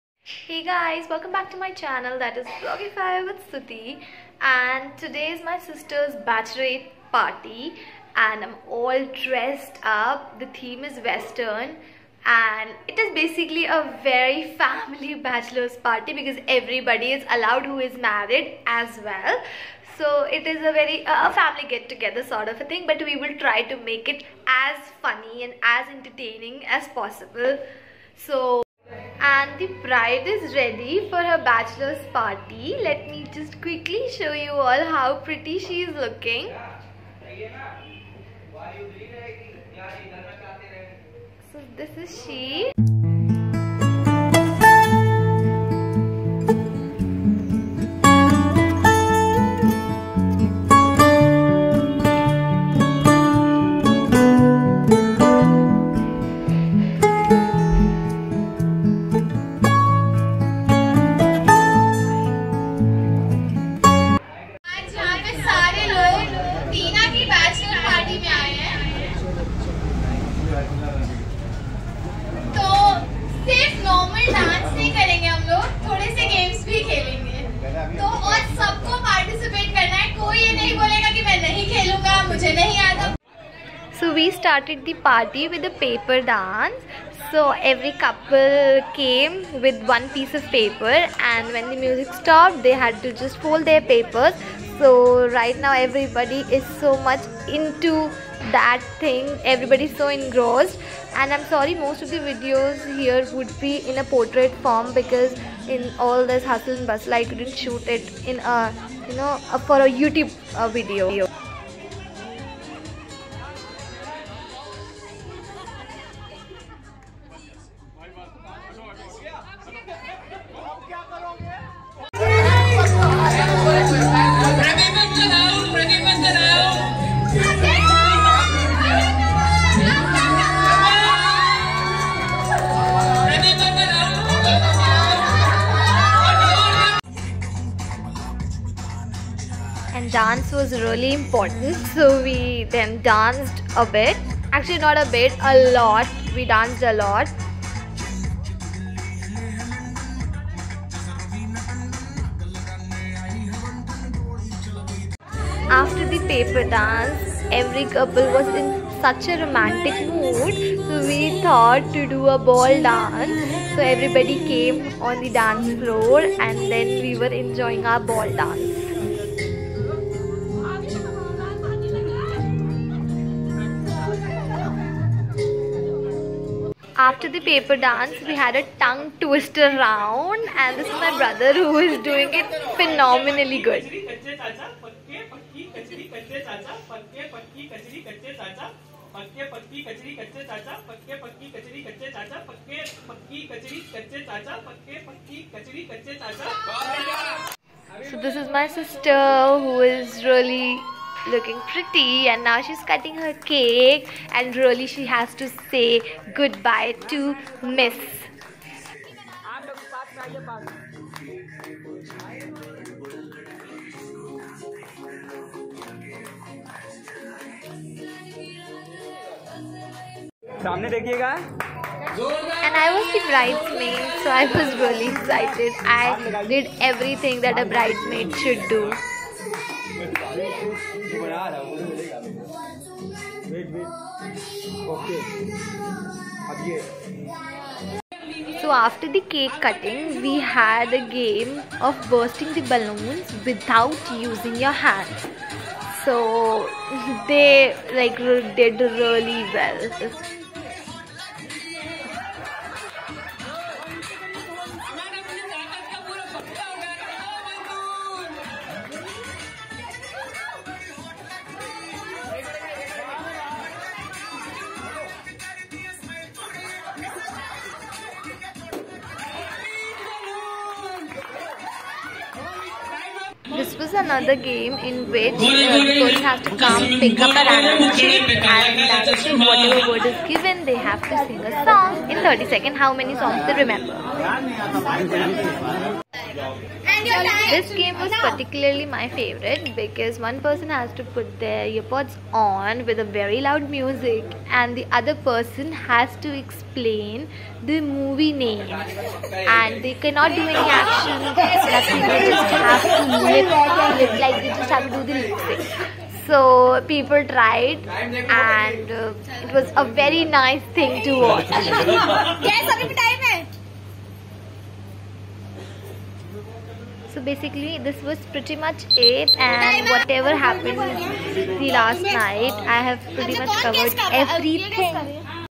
My sister's wedding vlog ( part -3)
Can't keep calm it's my sister's bachelor's party